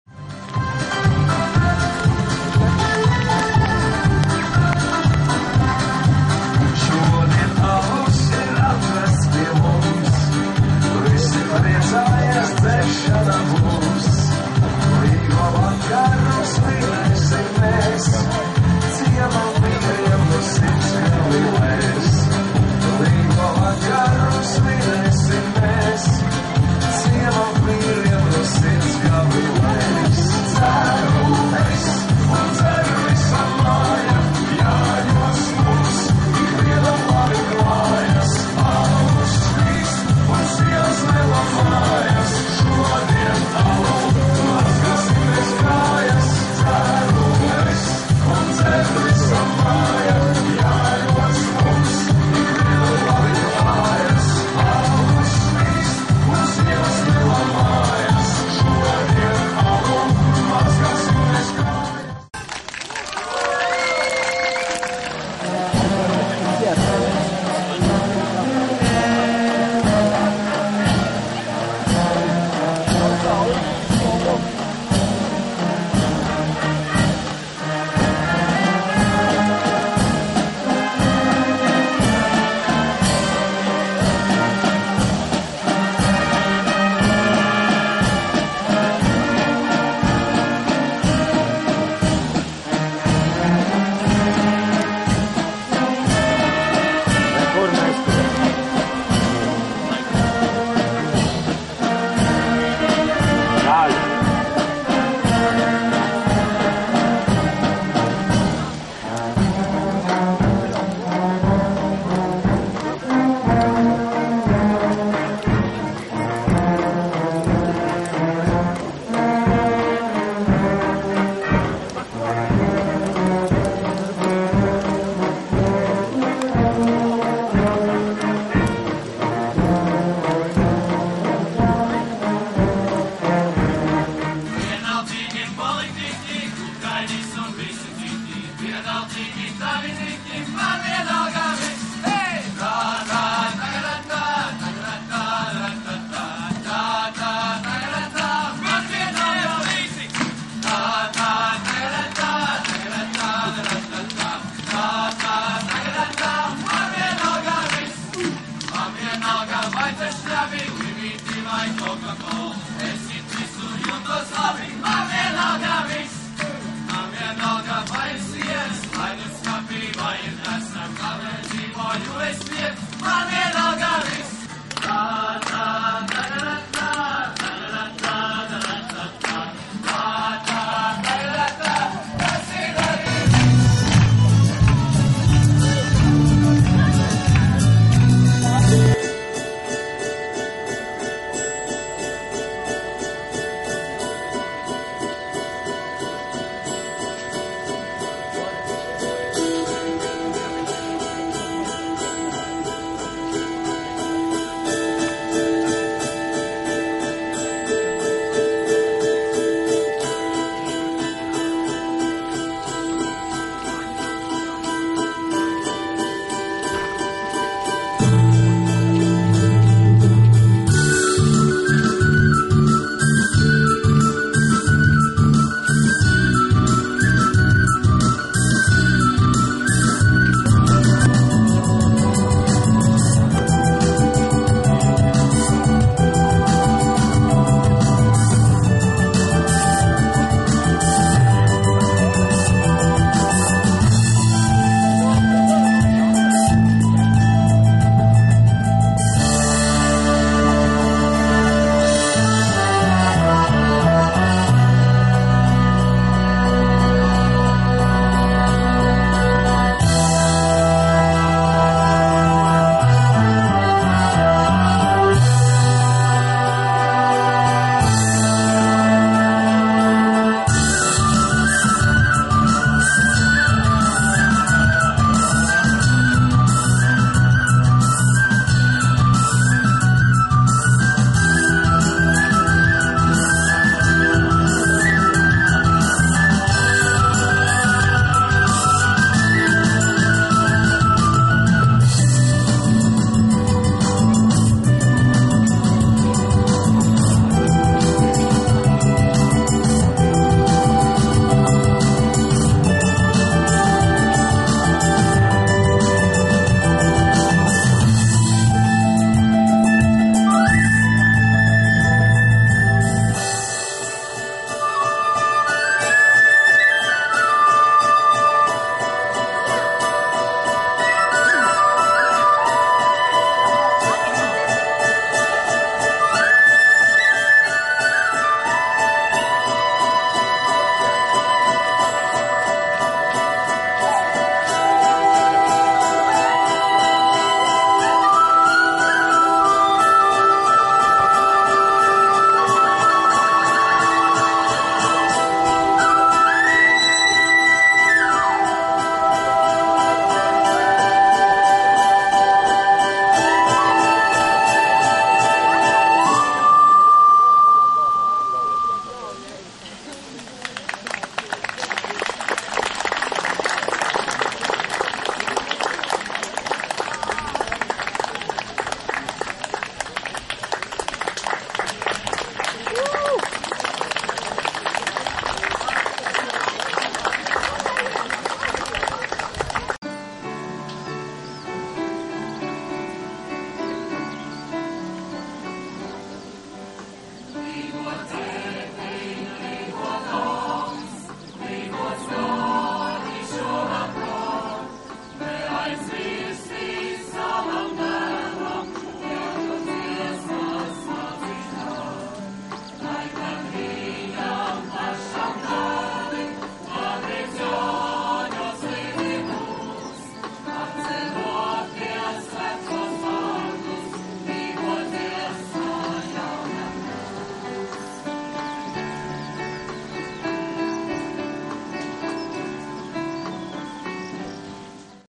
Лиго-2011.
Не дожидаясь официальной даты празднования Лиго 23 июня,в Латгалии начали отмечать уже 19 июня.Латвия,Вилянский край,Декшары.